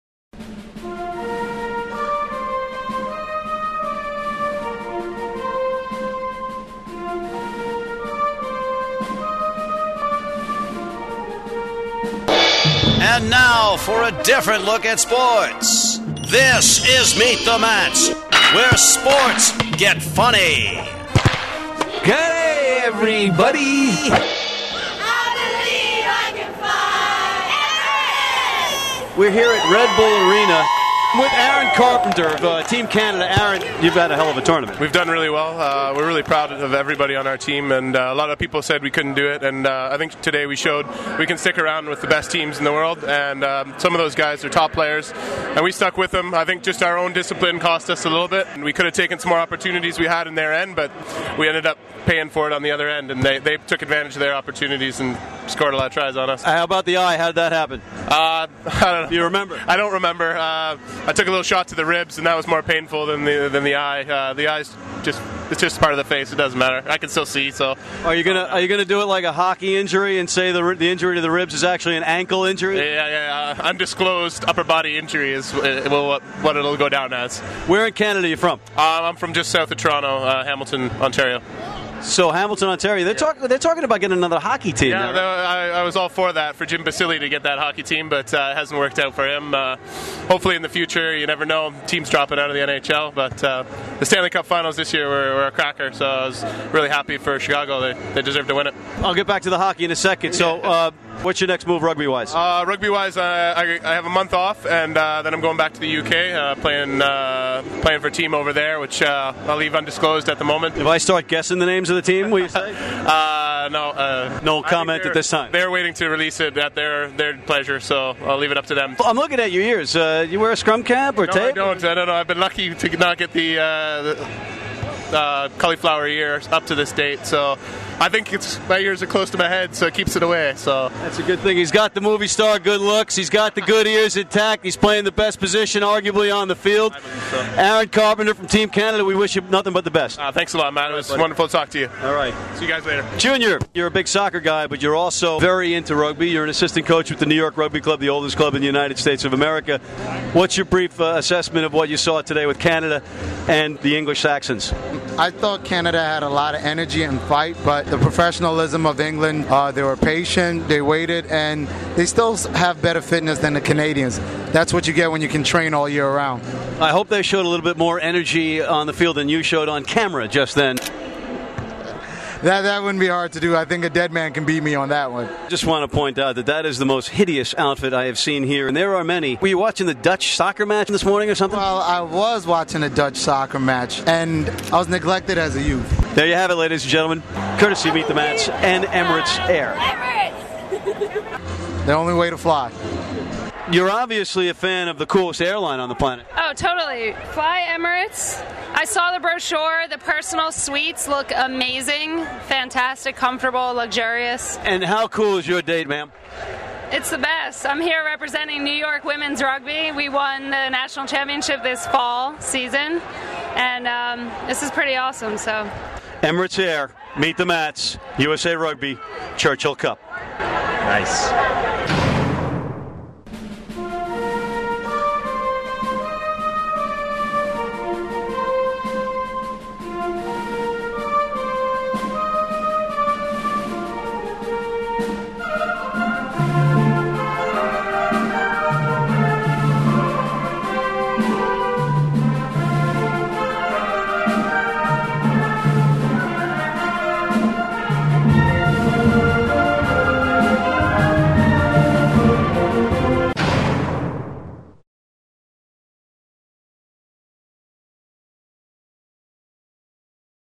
Rugby Canada's Aaron Carpenter, Rugby Rain Man Junoir Blaber, New York Rugby's Saburah Posner
Matt McCarthy, who we now share with RugbyWrapUp.com, interviewed Rugby Rugby #8 Aaron Carpenter, National Women's
Champion Flanker Saburah Posner of the New York Rugby Club and Rugby Wrap Up's Rugby Rain Man, Junoir Blaber.